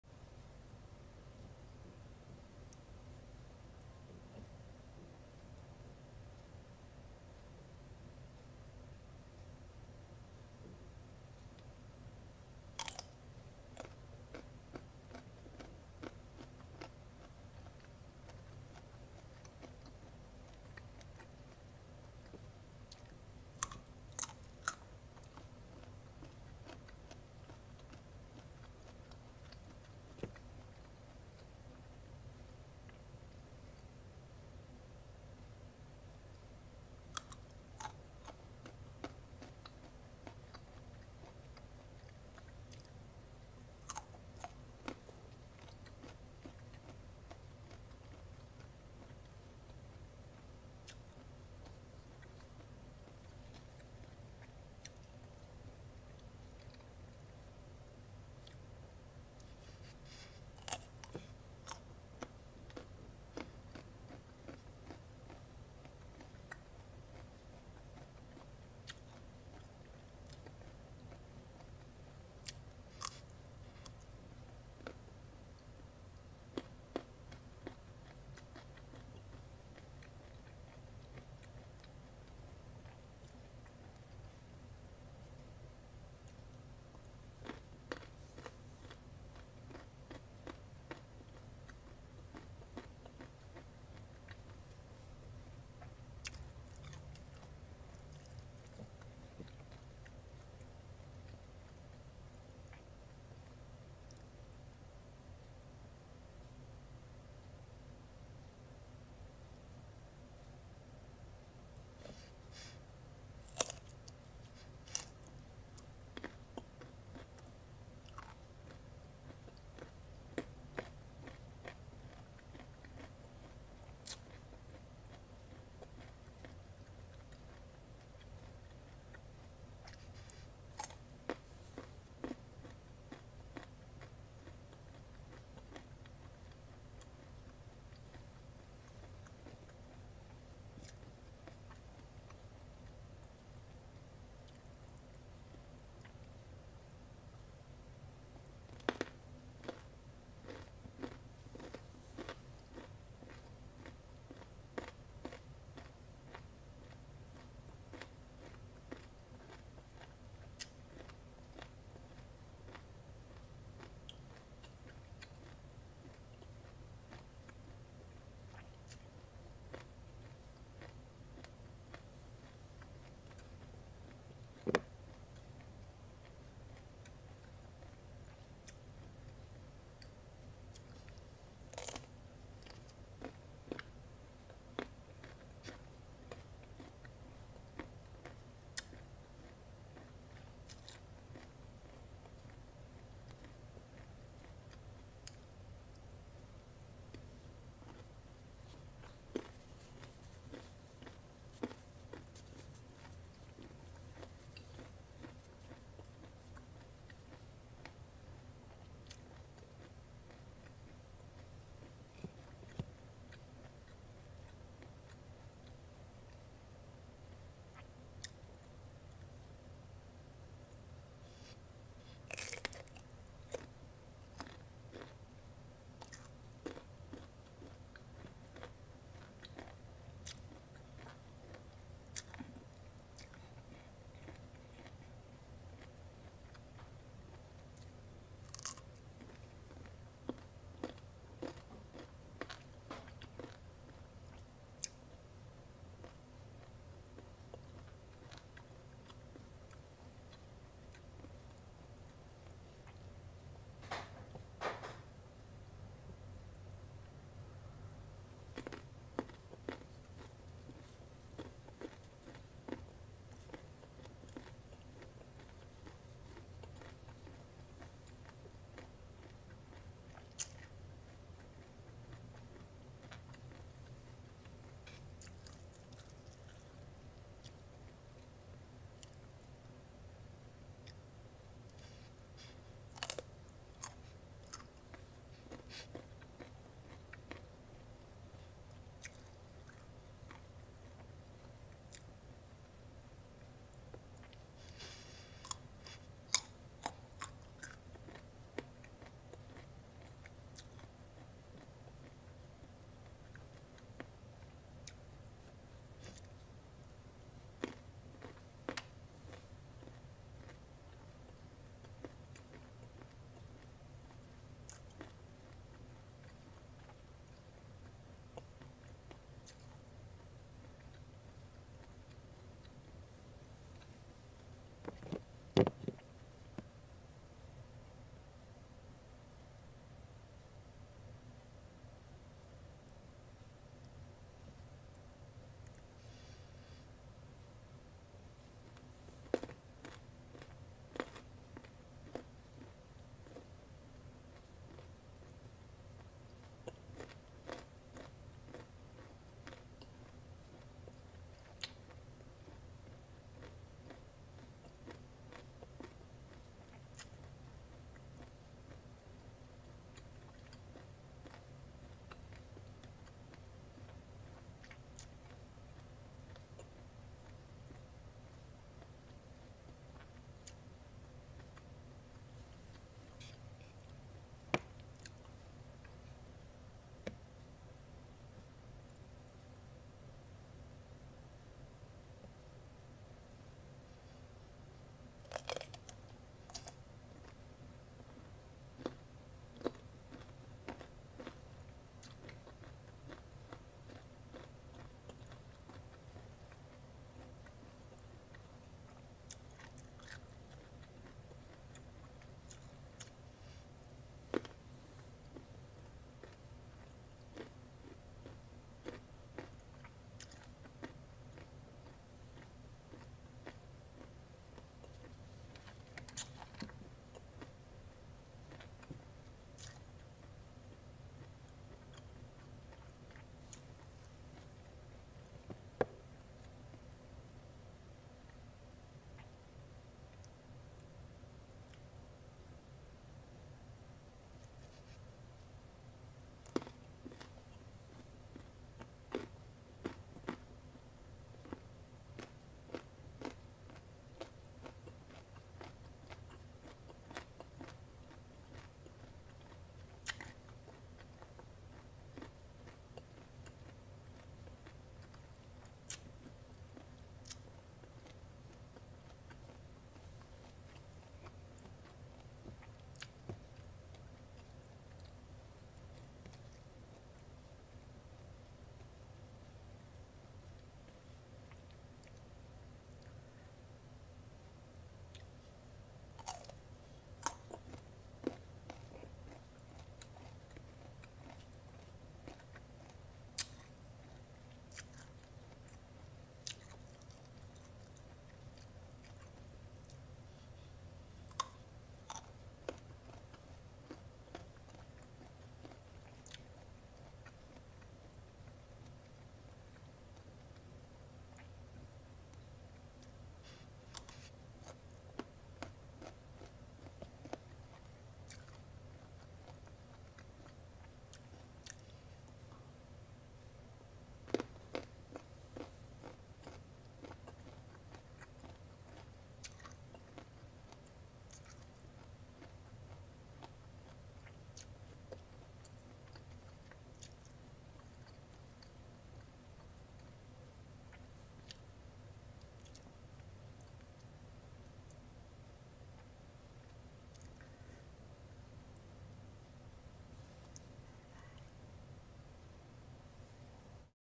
#04 asmr baby mango with source
Xoài xanh giòn rụm cùng với nước chấm thần thánh, chua chua ngọt ngọt cay cay, ngon tê tái!